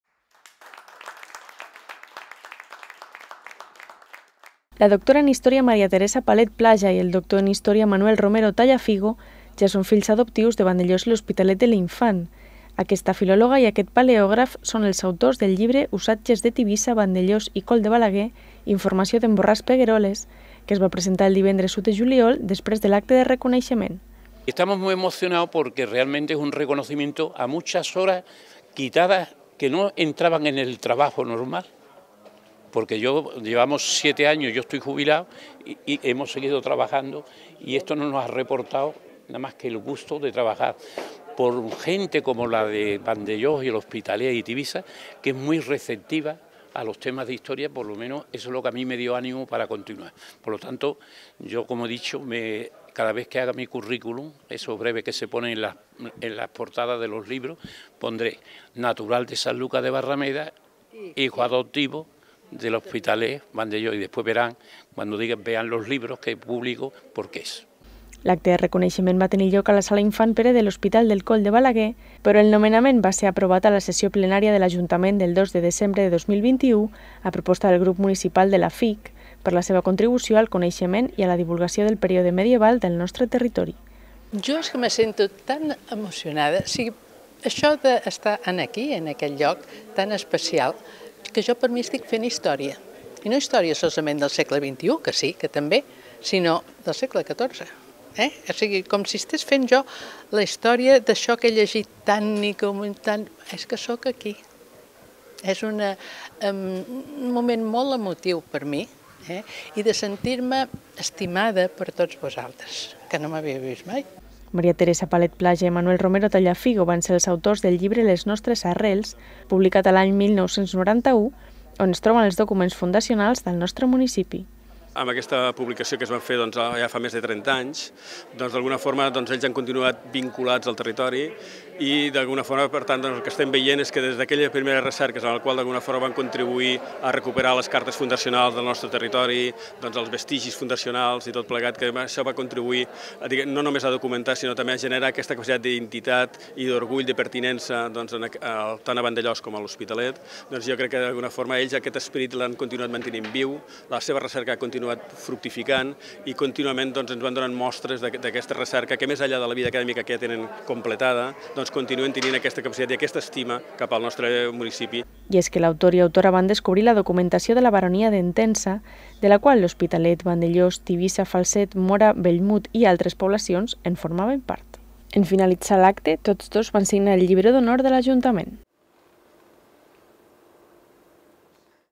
Una filòloga i un paleògraf, fills adoptius del municipi
La filòloga Maria Teresa Palet Plaja i el paleògraf Manuel Romero Tallafigo han rebut el títol de fills adoptius de Vandellòs i l’Hospitalet de l’Infant en un acte celebrat aquest divendres, 1 juliol.